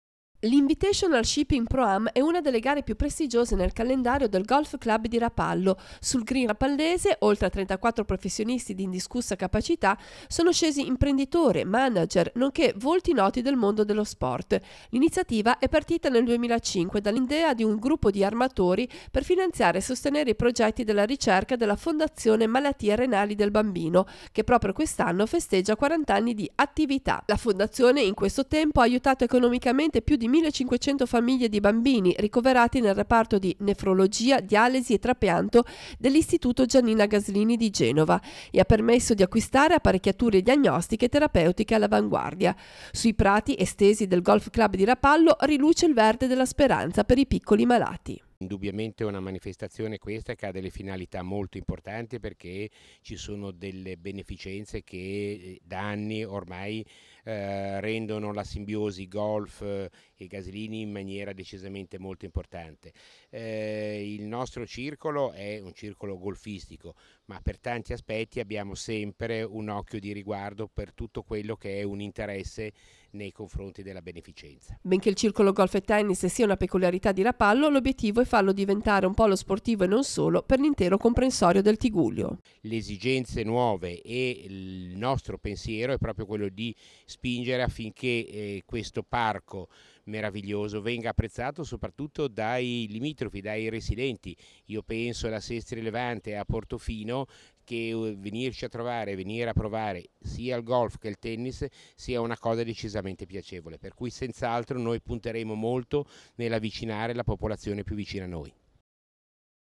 Trofeo Pro Am al Golf Club di Rapallo
Al Circolo Golf di Rapallo si è disputata la XIII edizione del Trofeo Pro Am, il cui ricavato è stato devoluto alla Fondazione malattie Renali del bambino, che fa capo all'istituto Giannina Gaslini di Genova. Un parterre di giocatori d'eccezione ha consentito di raccogliere oltre 110mila euro